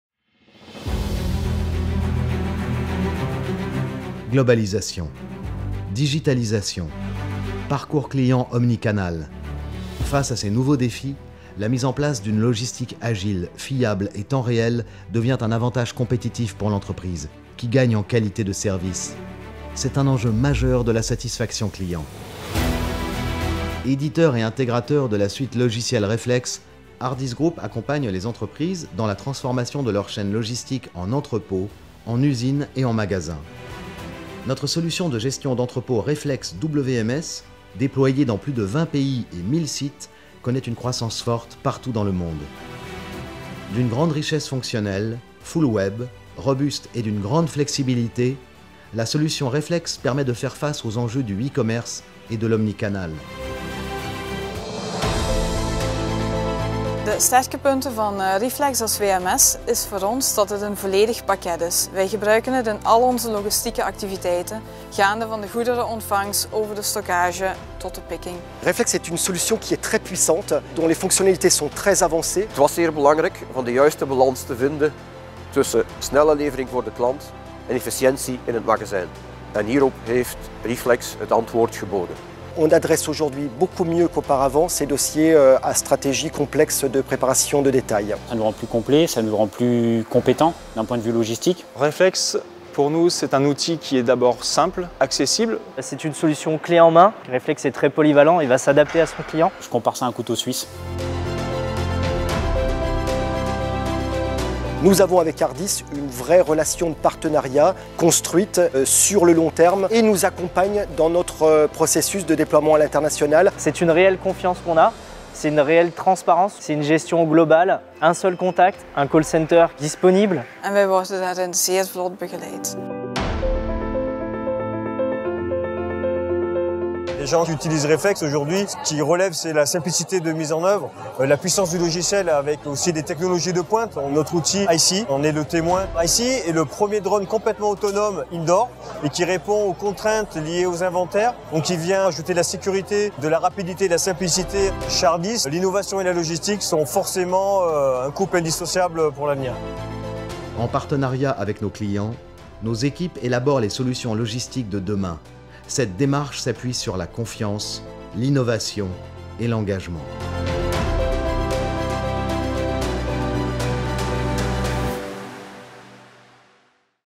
Reflex - La solution logistique d'Hardis Group
Reflex : suite logicielle de gestion logistique modulaire

Notre suite Reflex apporte une réponse globale aux enjeux logistiques des entreprises dans leurs usines, entrepôts et points de vente. Elle contribue à vous faire gagner en performance, qualité de service et réactivité, afin de répondre aux attentes de clients de plus en plus exigeants, internationaux et connectés.